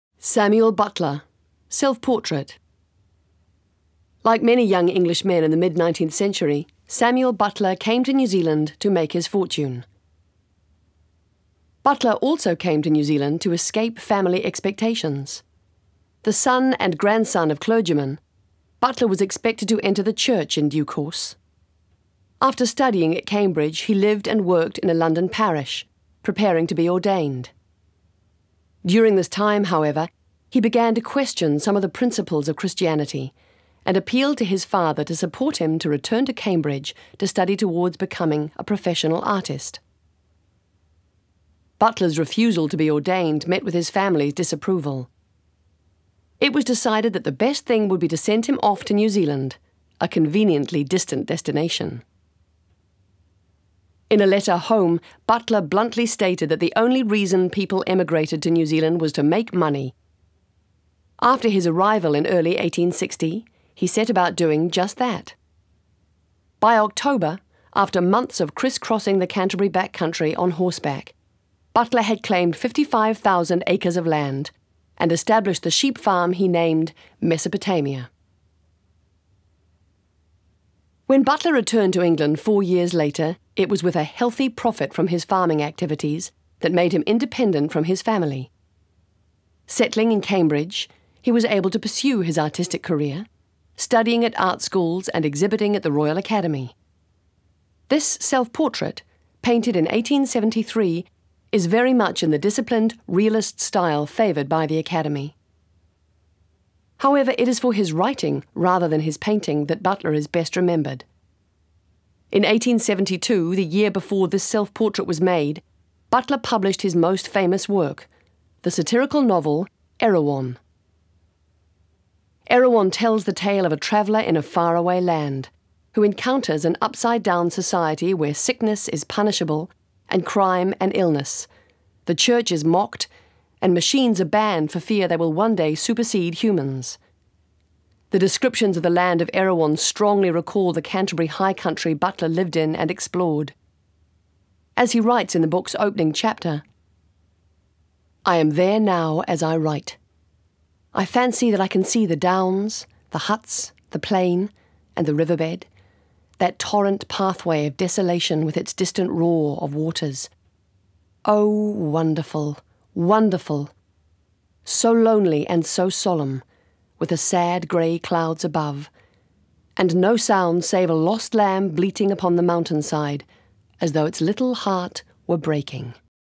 Samuel Butler - Self Portrait (1873)
An introduction to Samuel Butler's Self Portrait (1873), narrated by New Zealand actor Miranda Harcourt.

Samuel Butler
British, b.1835, d.1902
Self Portrait

Presented by the Canterbury College Board of Governors, 1933
820 x 720 x 65mm
69/76
1873